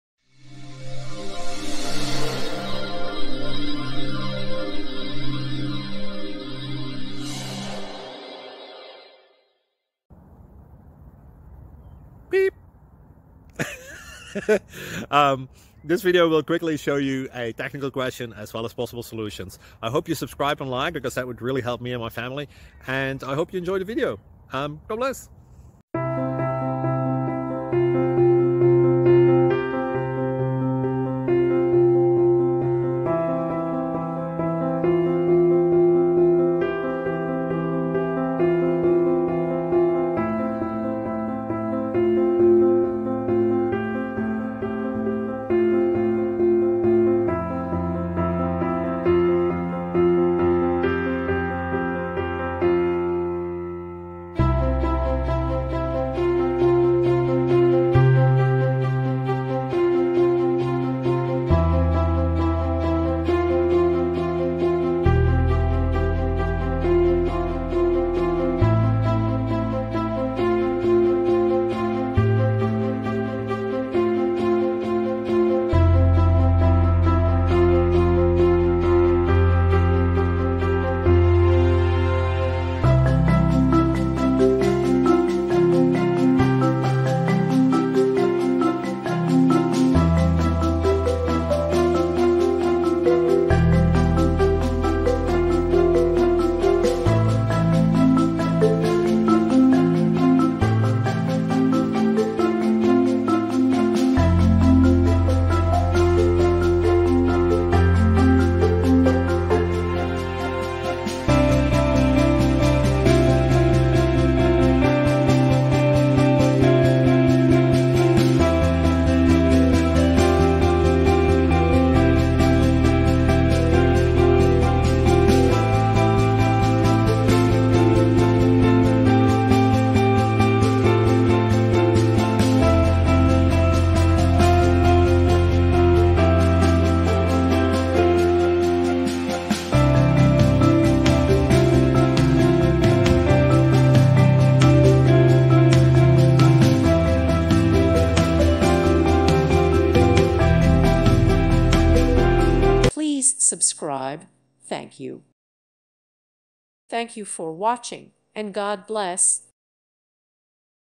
Sorting by checking 2 distance away elements in a list, and check the method's accuracy
Code Review: Sorting by checking 2 distance away elements in a list, and check the method's accuracy

With thanks & praise to God, and with thanks to the many people who have made this project possible! You are responsible for your own actions.